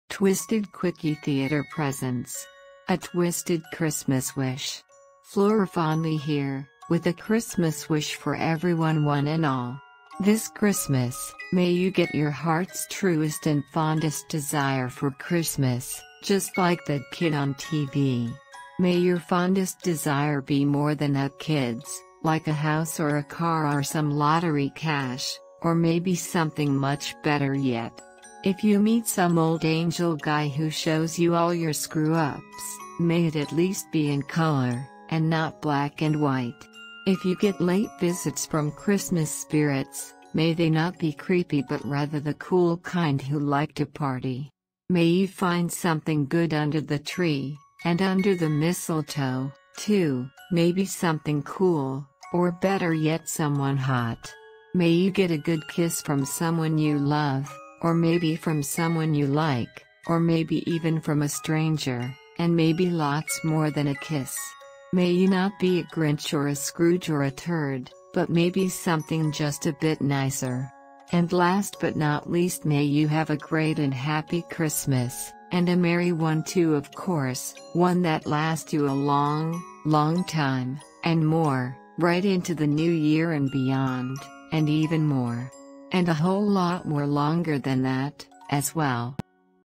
A Twysted Christmas Wish
Just in time for the Big Christmas Holiday, Twysted Quickie Theatre Hostess Flora Fondley brings us a Christmas Wish for everyone one and all, with just the right seasoning of mischievous fun, of course!